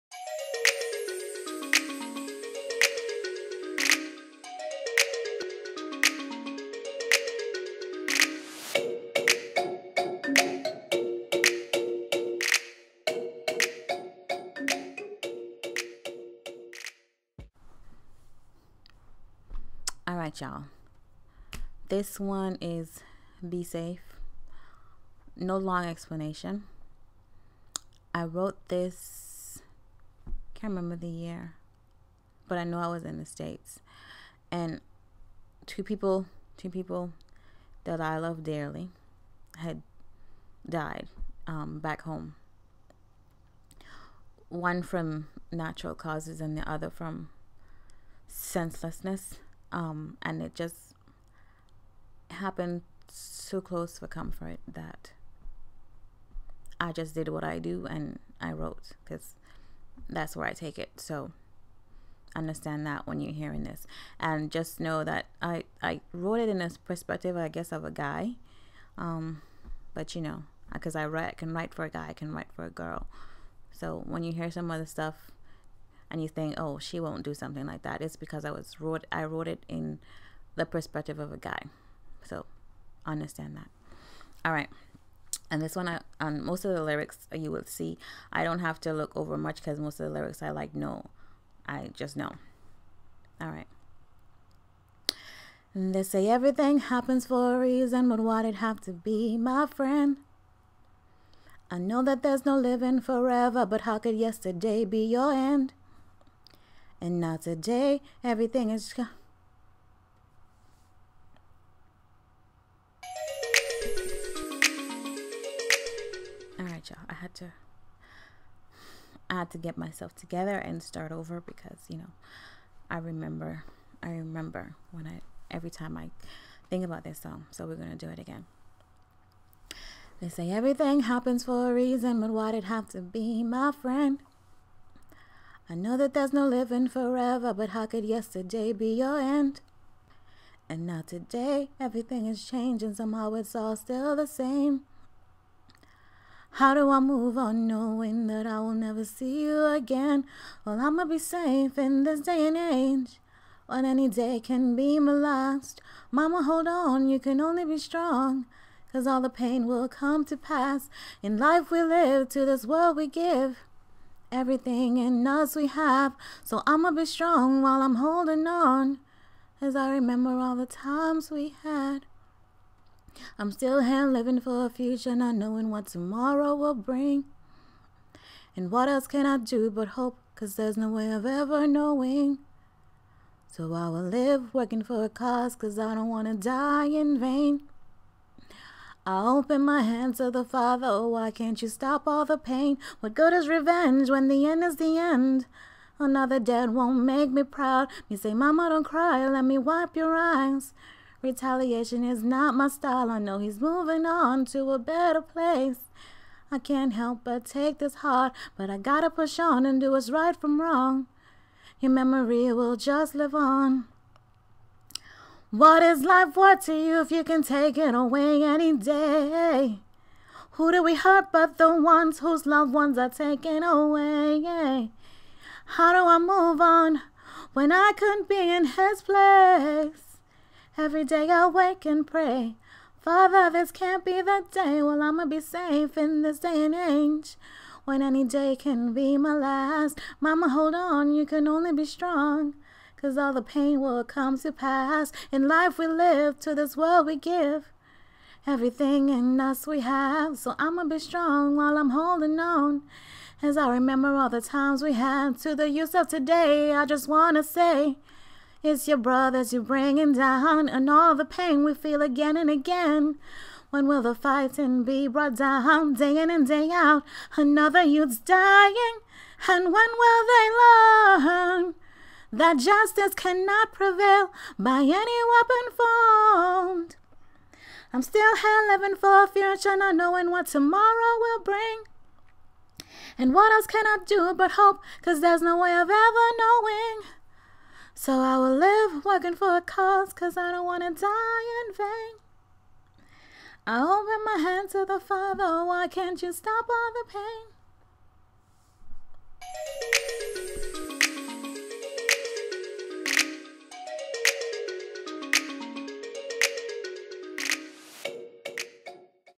Be Safe - Words and Lyrics S1: E4
Writing in times of tragedy is hard, but also deep. I realize that most artists who are soulful or are real lyricists have all been through something in their lives to allow them to write that deep and thoughtful. Or at least that's what I think anyway. What are your thoughts?

UPDATE! - You can now find these words in my first poetry collection, "Black and White Composition: The Rhymings of a Young Poet".